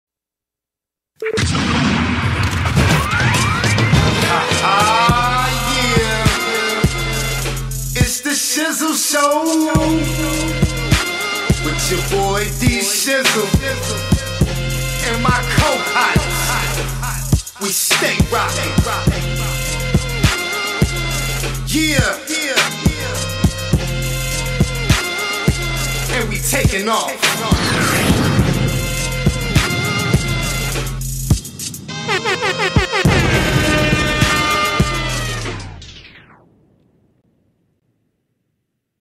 The Shizzle Show Episode 4 12-2-18
Executive Studios representatives and friends stop by to kick it with us.